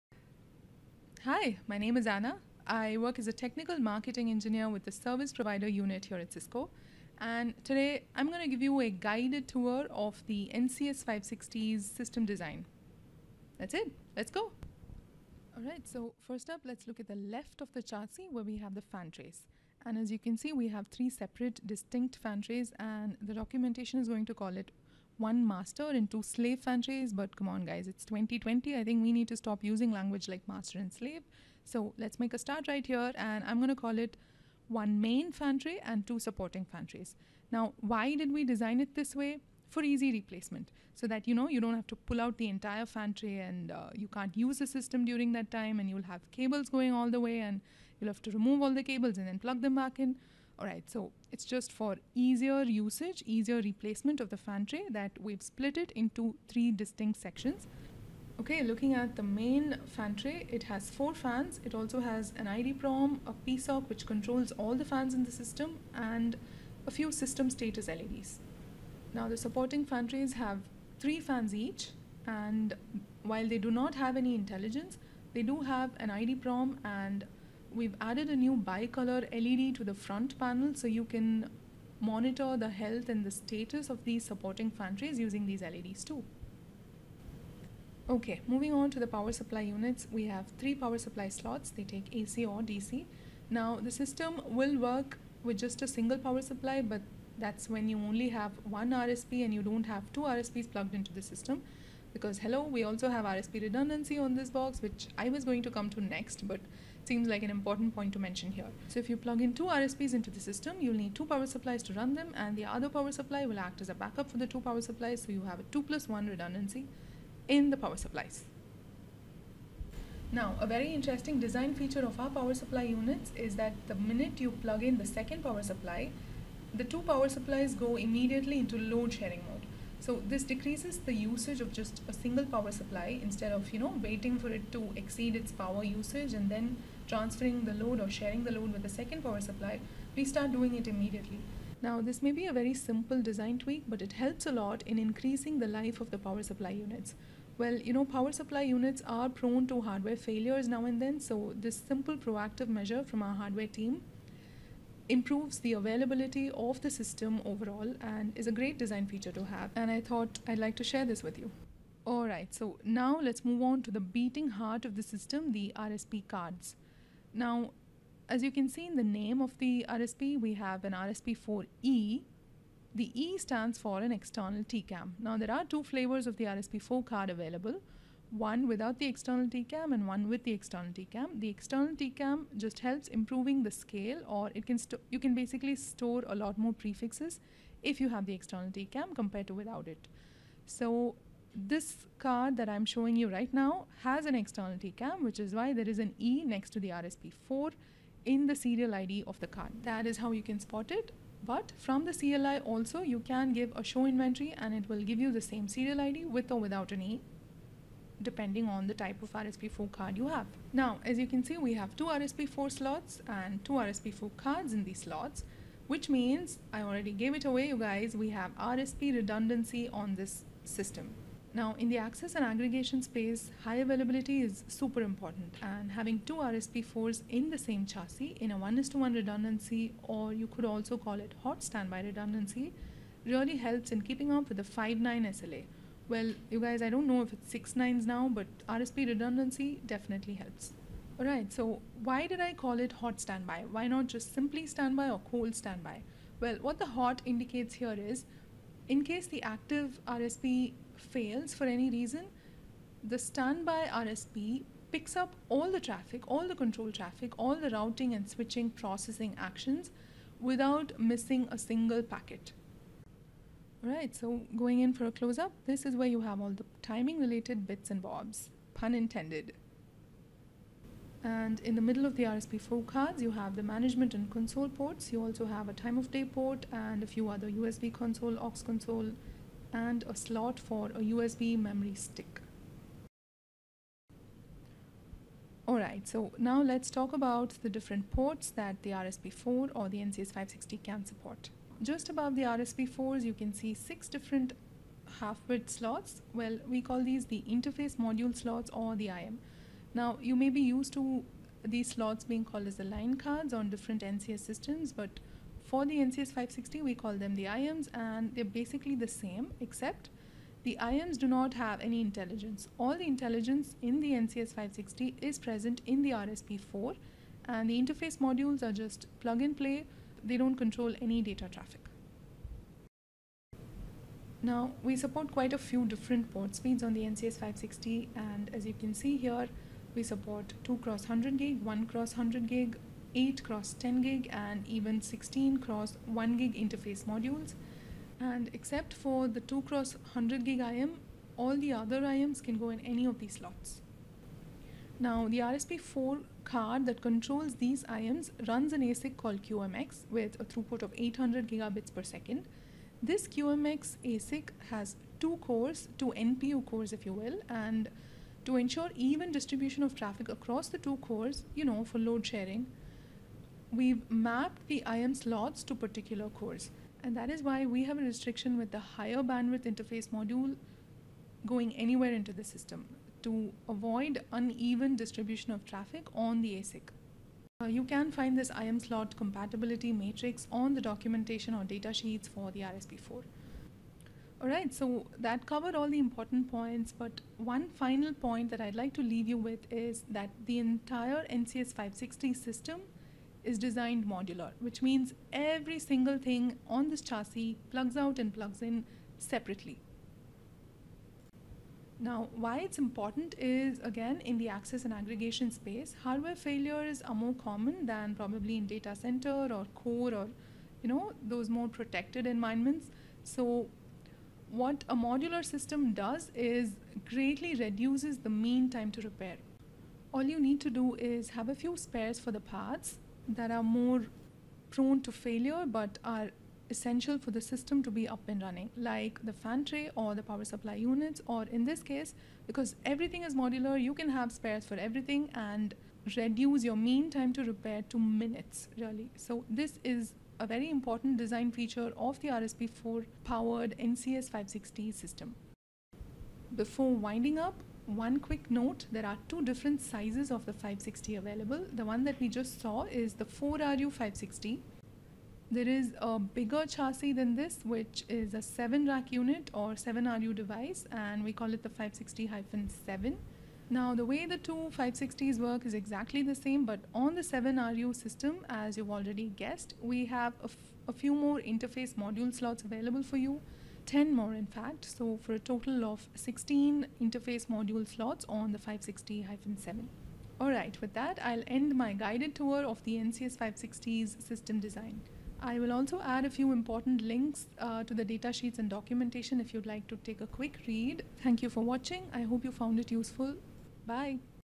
In-depth NCS560 system design
Understanding NCS560's system design is key to learning the unique demands of access/aggregation routers & how Cisco's portfolio of NCS routers stands out head & shoulder's above the competiton.